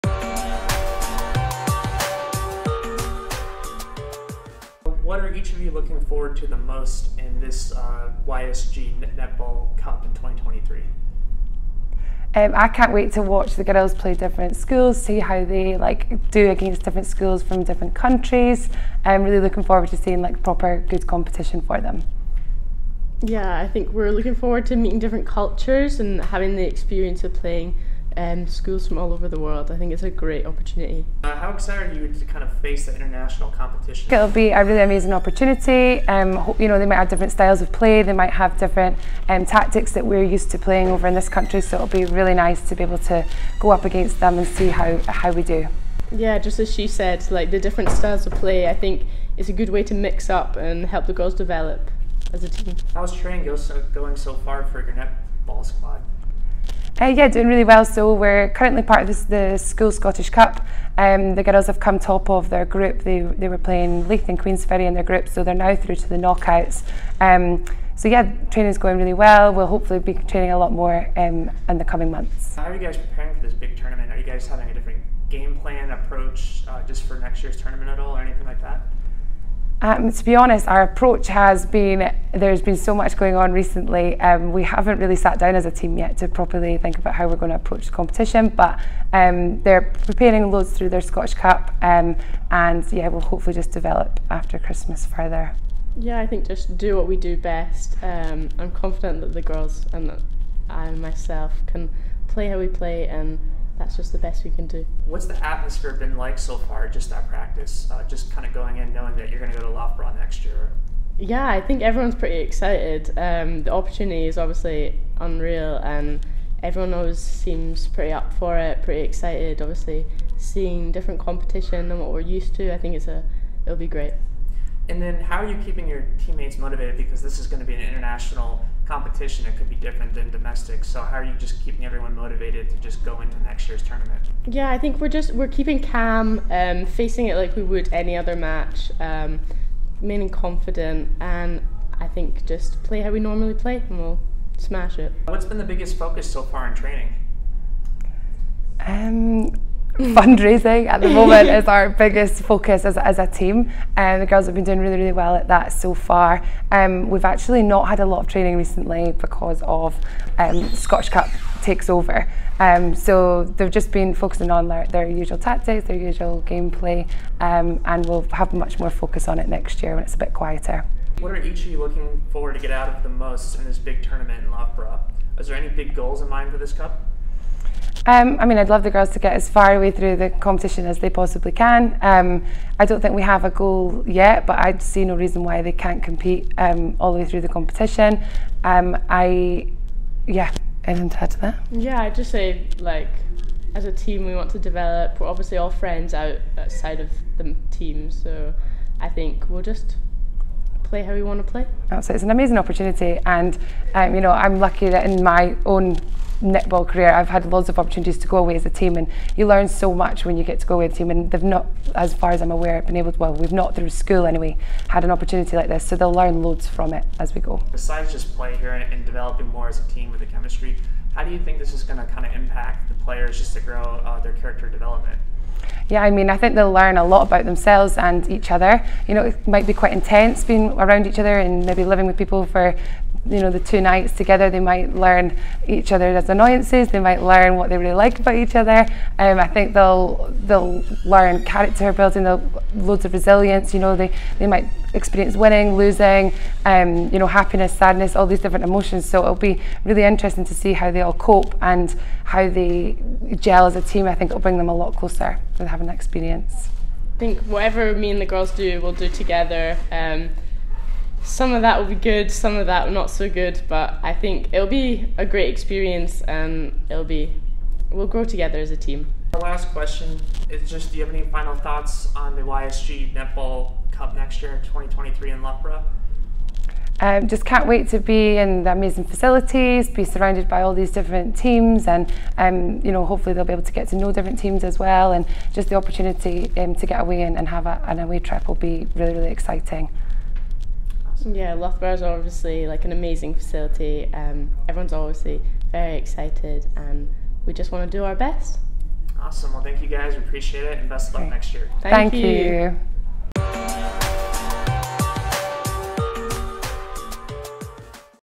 WSGNetball - Tune in With the Team: Balerno High School
𝗘𝗫𝗖𝗜𝗧𝗘𝗗 𝗧𝗢 𝗖𝗢𝗠𝗣𝗘𝗧𝗘 🏴󠁧󠁢󠁳󠁣󠁴󠁿
We went down to Balerno High School in Scotland to see how excited the school's netball team is for the upcoming WSGNeball Cup 2023.

Watch above to hear what Coach Hannah and her star captain had to say about the international competition at Loughborough University.